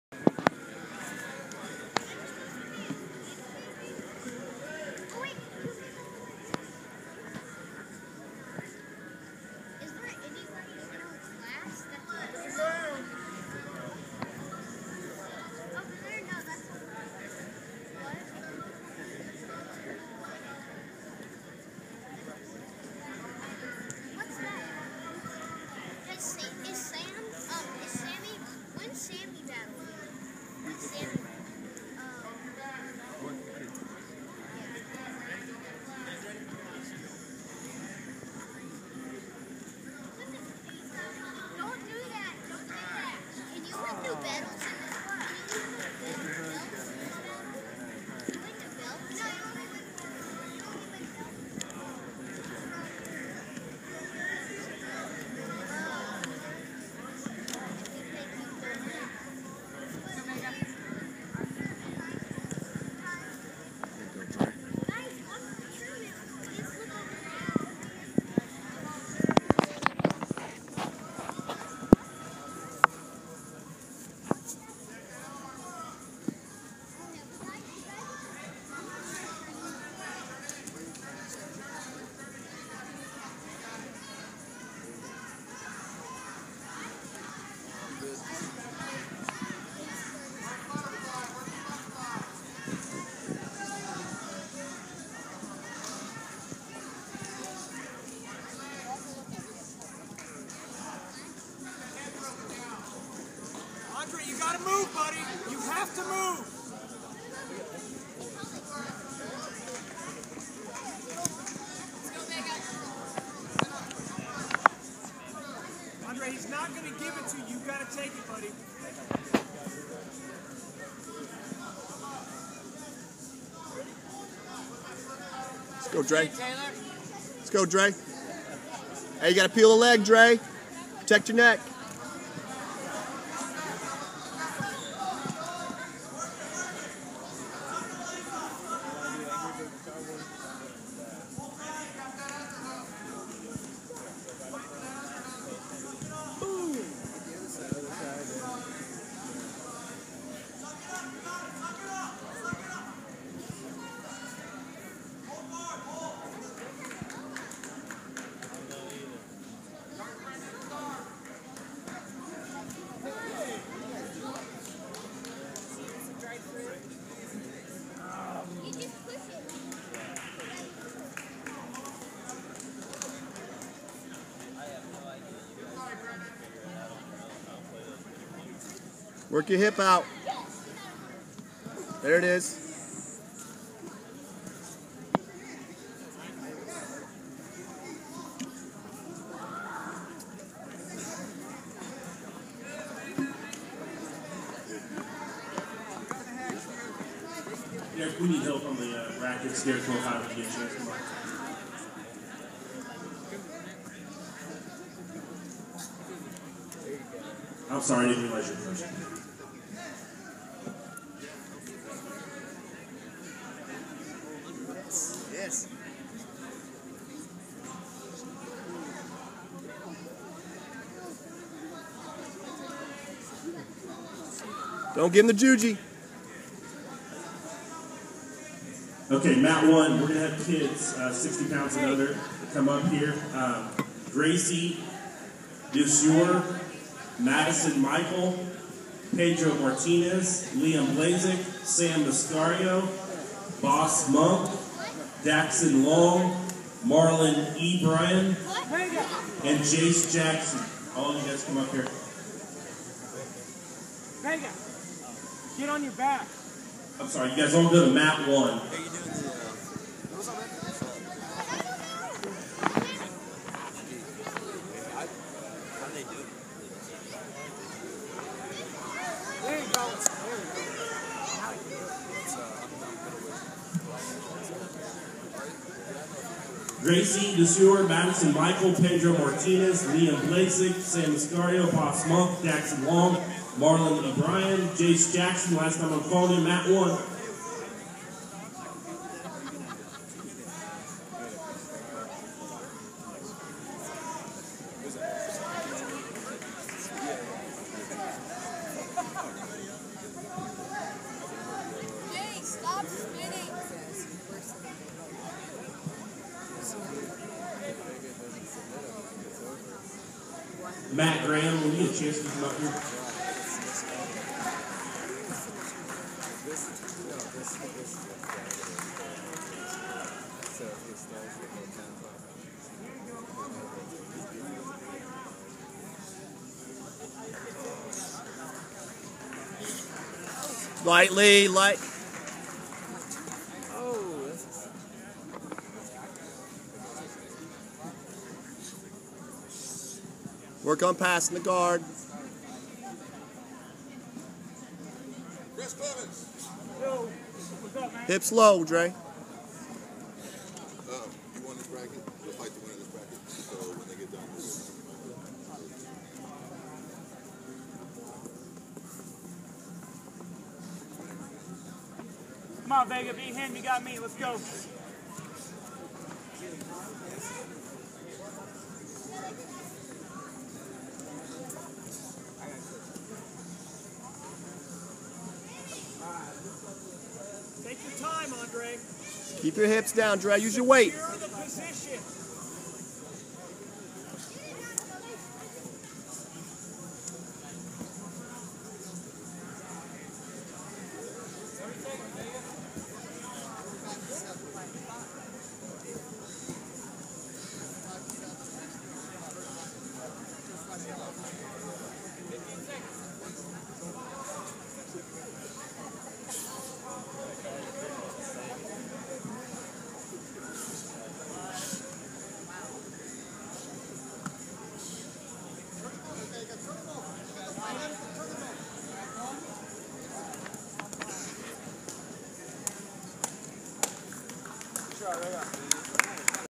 Black Rhino vs Adam Vega-No Gi Absolute Submission Only
KCMMA's Metro Submission Expo II, North Kansas City High School, Missouri - December 7, 2013